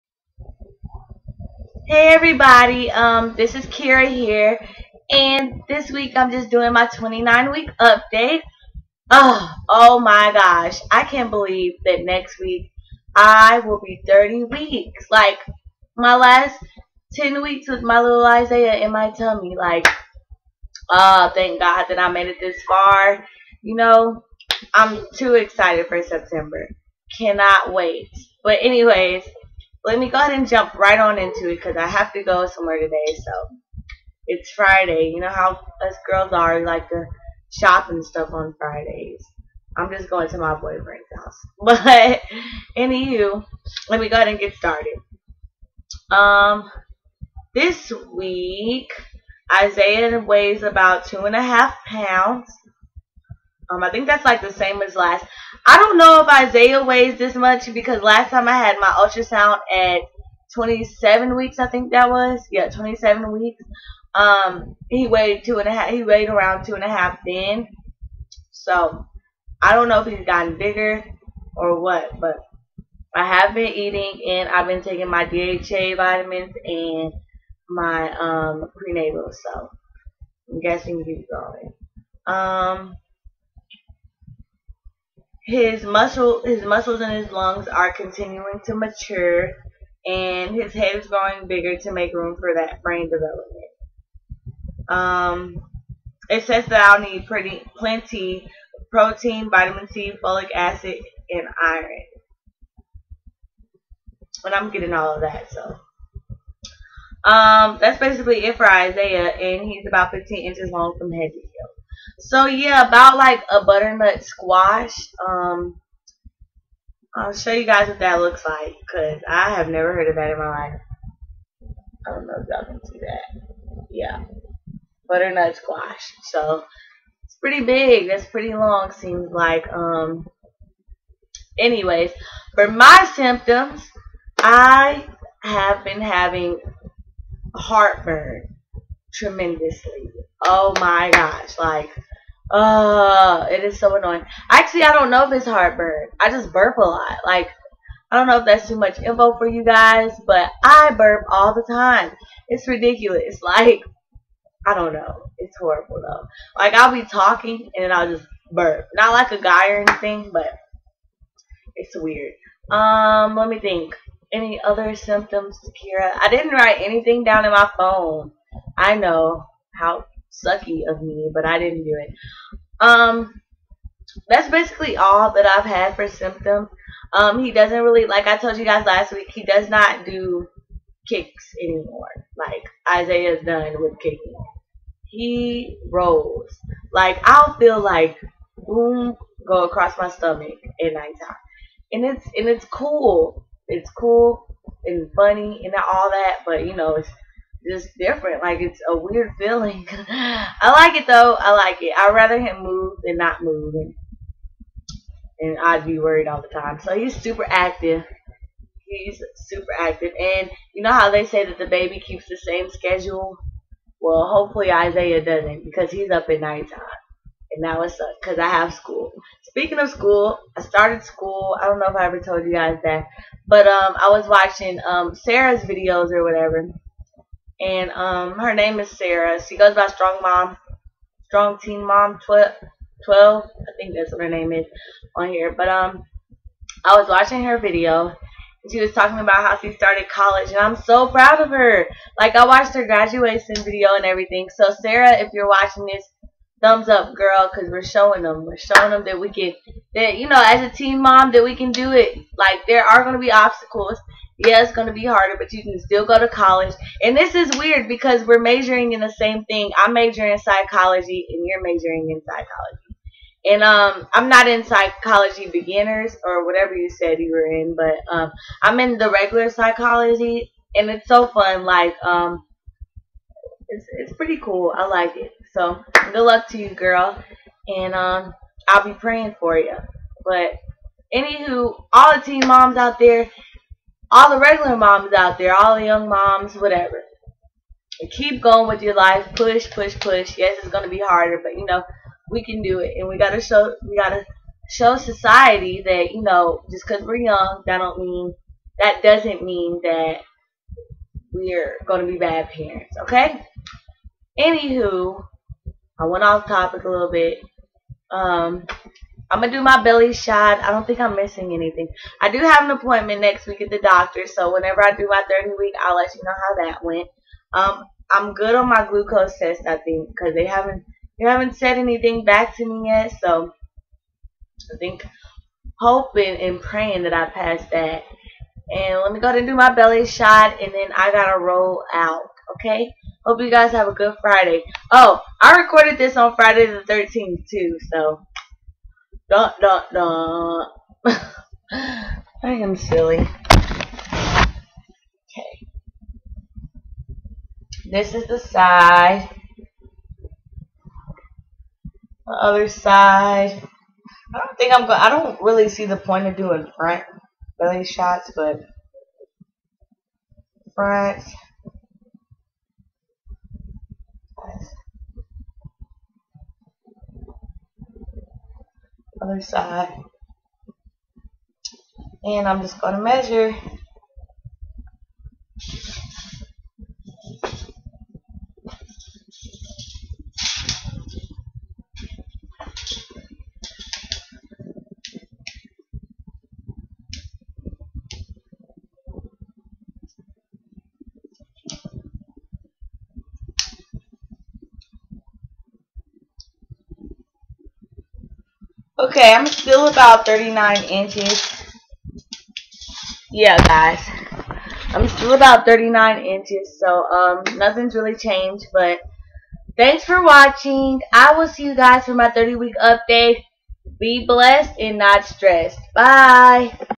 29 weeks pregnant & Belly Shot ! : )
Whew , Only about 10 1/2 more weeks too go ! YaY ! Hope you ladies enjoy : ) Sorry if It's kinda loud :/